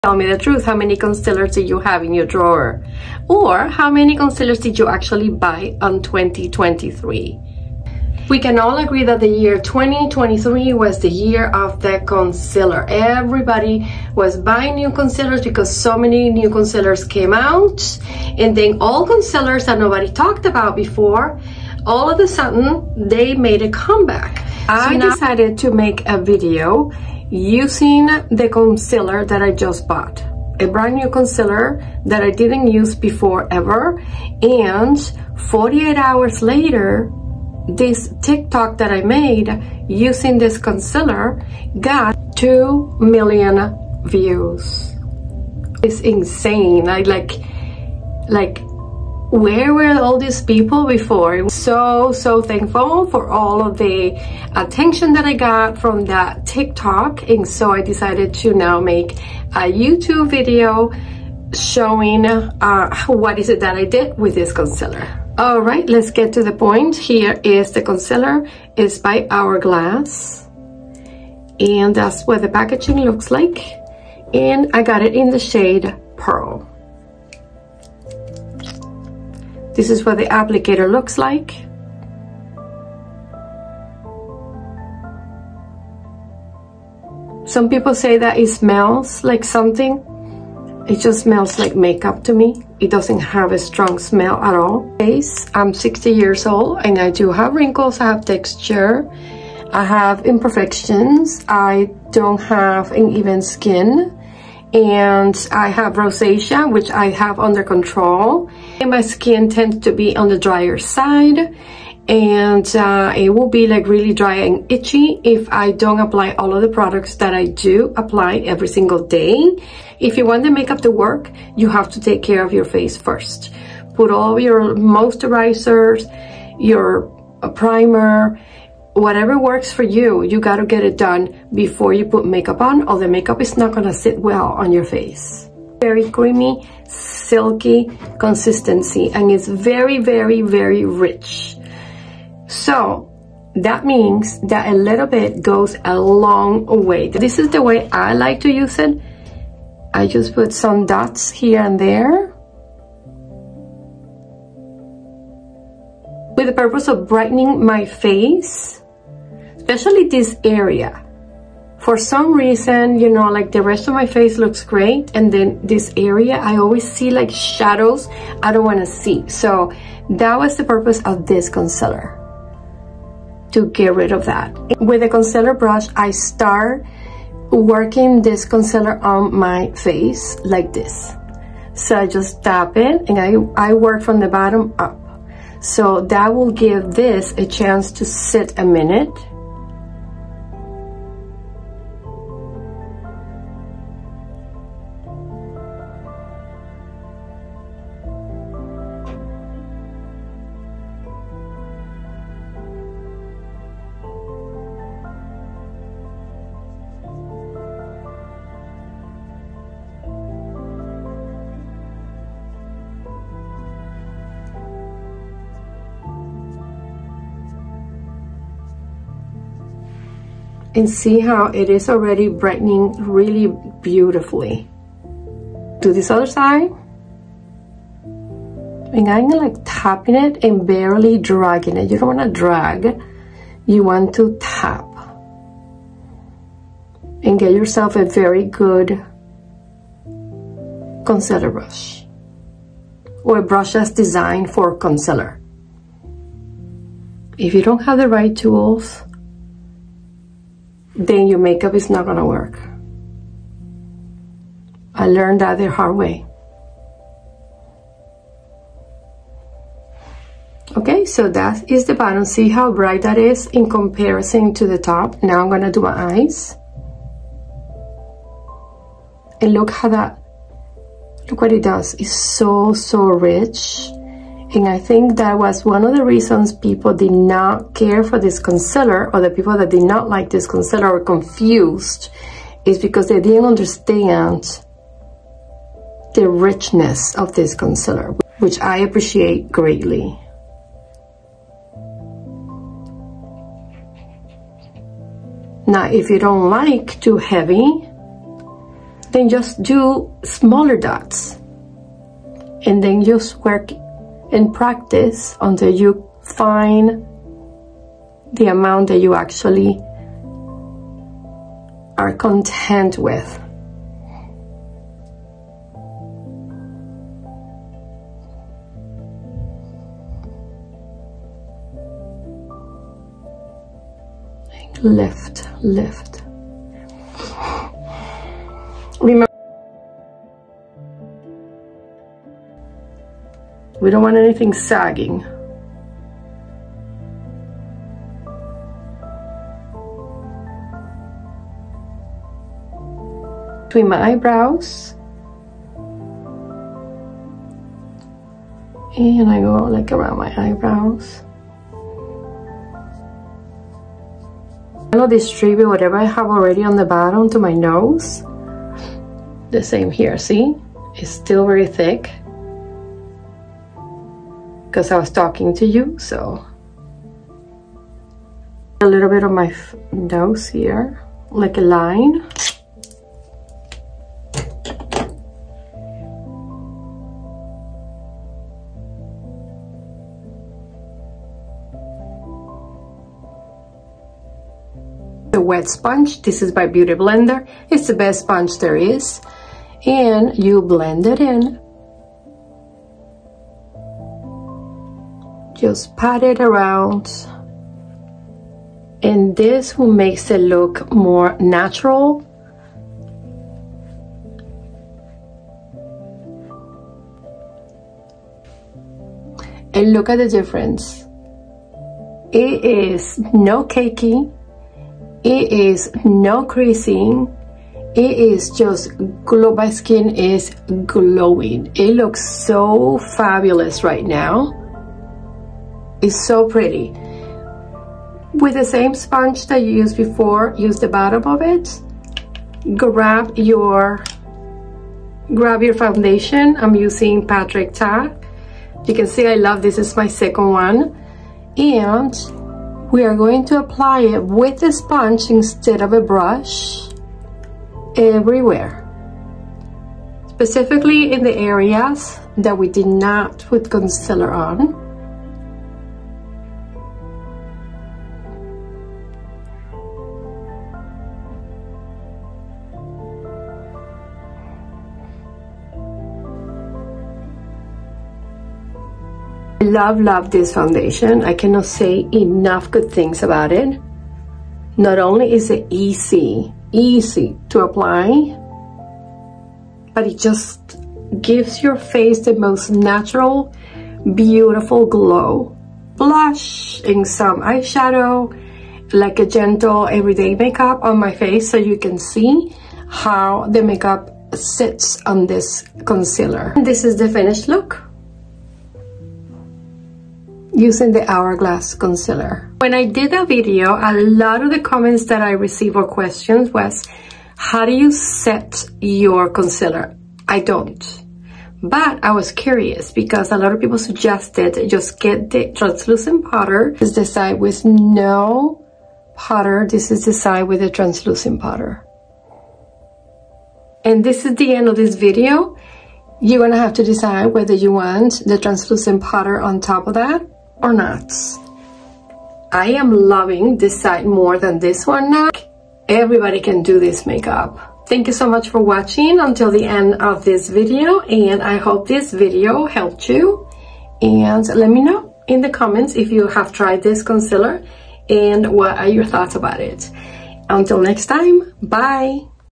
The 2 Million Views Concealer: Hourglass Vanish Airbrush Concealer
This concealer by Hourglass is absolutely fabulous! After I bought it, I purchased a couple of their other products and my mother in law gifted me the Ambient Snake Palette with the matching brush. So luxurious! Expensive but Hourglass’ products are super high quality and worth every penny in my humble opinion.